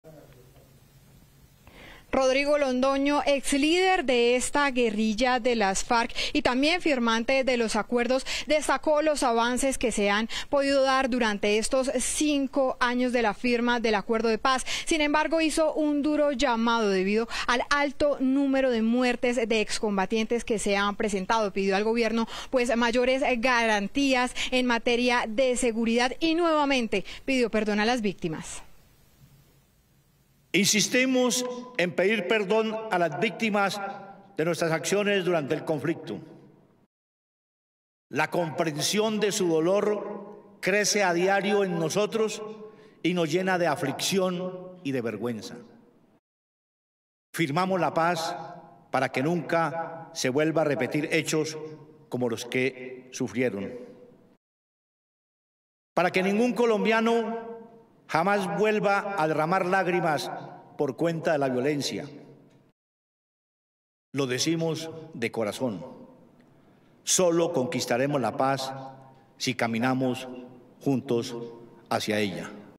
Rodrigo Londoño dice que el acuerdo de paz fue un acierto: “Puso fin a un conflicto armado”
Durante la conmemoración de los 5 años de la firma del acuerdo de paz con la guerrilla de las FARC, Rodrigo Londoño, excabecilla de ese grupo al margen de la ley, destacó los avances del proceso, pero también criticó el número de excombatientes asesinados.

Rodrigo Londoño aseguró que el proceso de paz fue la vía correcta para poner fin a un conflicto armado.
“Nada ni nadie podrá minar nuestra convicción de que el camino emprendido es el correcto. El acuerdo puso fin a un conflicto armado de más de medio siglo, fue un gran consenso histórico”, indicó el excabecilla.